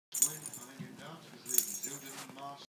Beautiful Playful Kitten for sale!
We would love our beautiful kitten to go to a lovely family. Great with Kids, very energetic and fun. Fully litter trained. £60 o.n.o.